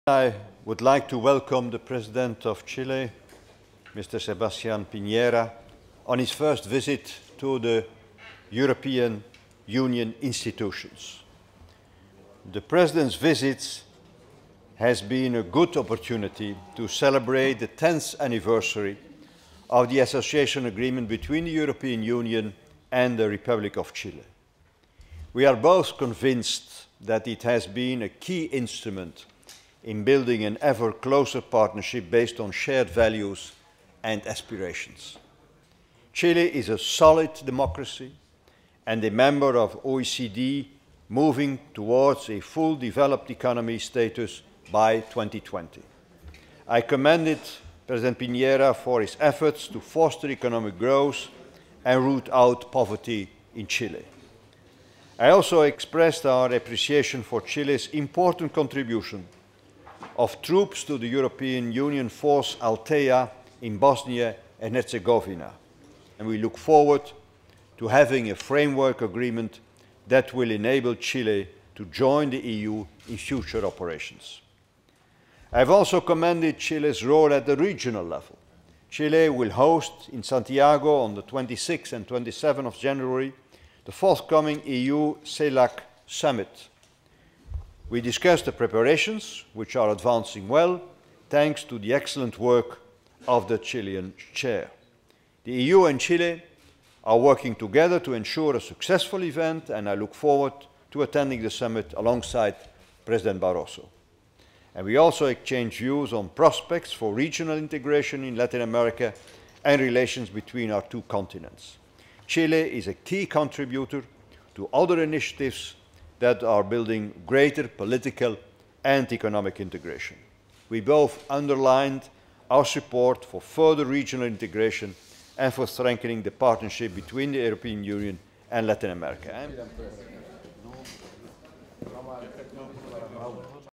Meeting with the President of Chile, Sebastian PIÑERA
Herman VAN ROMPUY, President of the European Council, meets Sebastian PIÑERA, President of Chile, in Brussels.